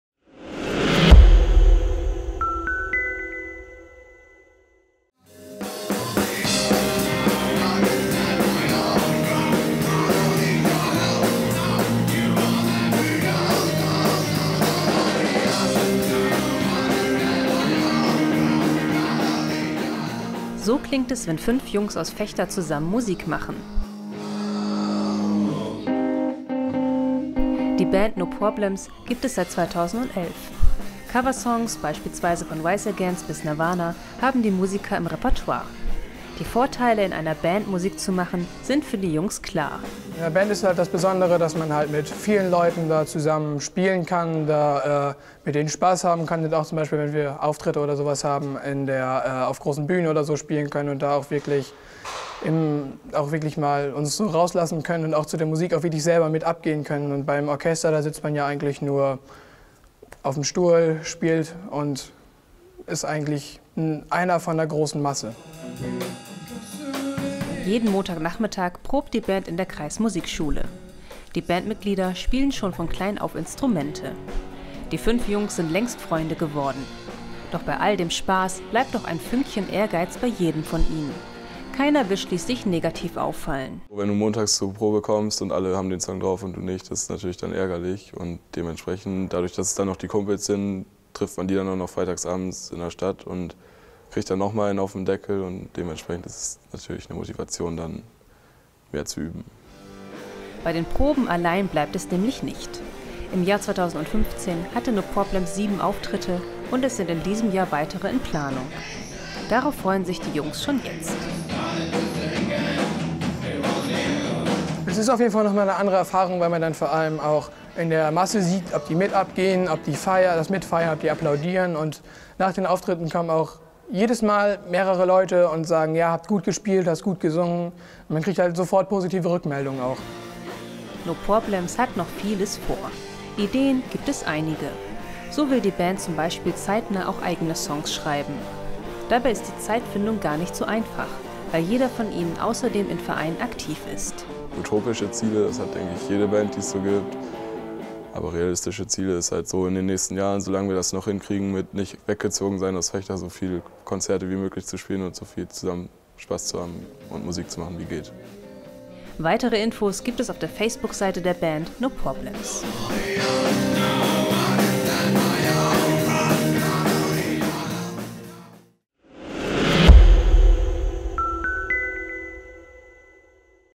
Musikserie Teil 2: No Problems aus Vechta
"Wir machen Musik" - heute mit Teil 2 unserer neuen Serie. Wir stellen euch die Band No Problems aus Vechta vor. Weitere Infos zum Thema gibt es in der OV von Dienstag (10.05.) und im epaper.